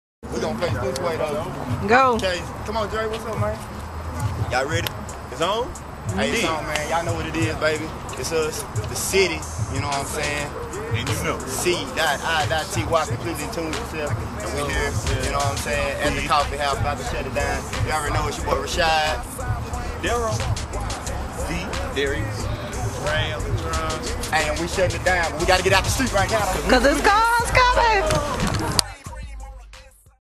The Streets are Watchin... and vice versa
Worst place to do an interview. THE MIDDLE OF THE STREET ! Loooook ooouuuttt lol
( NOTICE how many times they look @ the cars ... nervously lol  )
SHOUTS OUT to DORROUGH MUSIC...